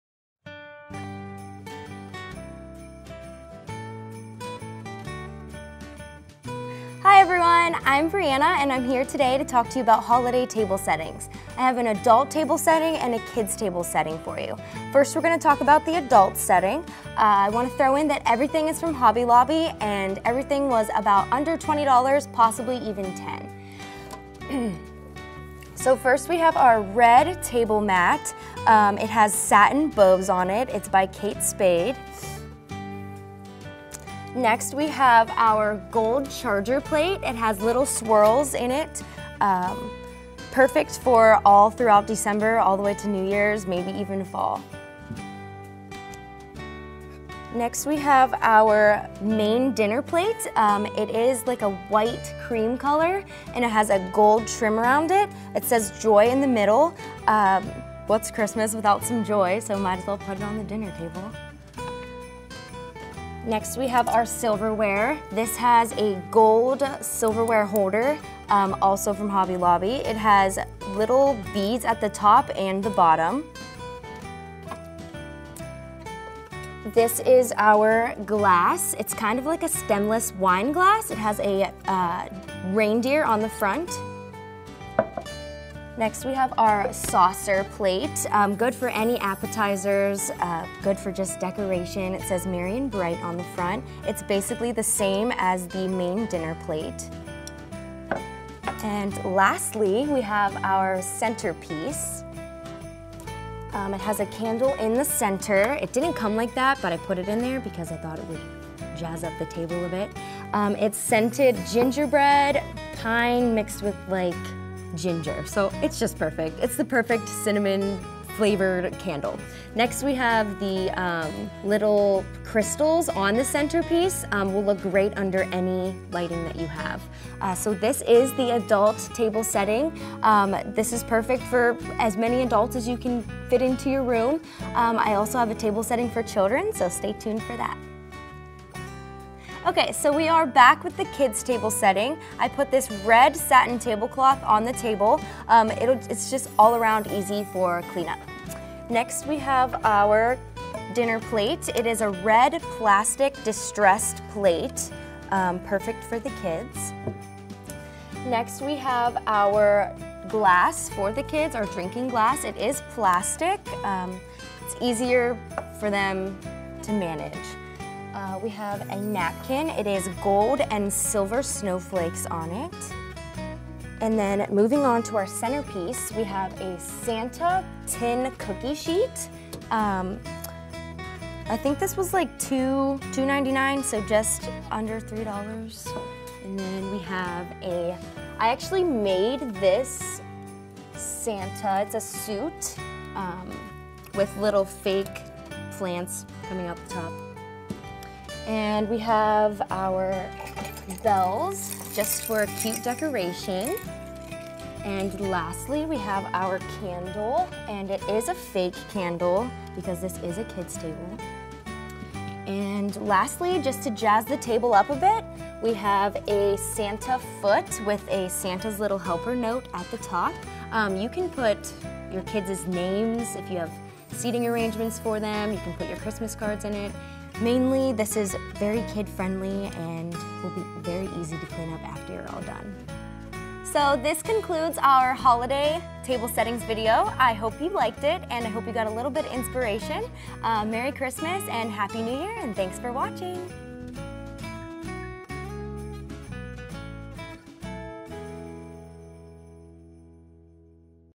'Tis the Season for Table Settings
Welcome family and guests in to your home and invite them to also to take a seat at your table while being surrounded by festive decor that is surely to get everyone in the holiday spirit.  Our blogger Bre shares how to 'tis the season for table settings for both an adult table or kid's table, perfect for your holiday party.  Strive to be the ultimate holiday hostess this year and serve the food on Christmas cheer!

All items were purchased at the Eastwood Mall Complex.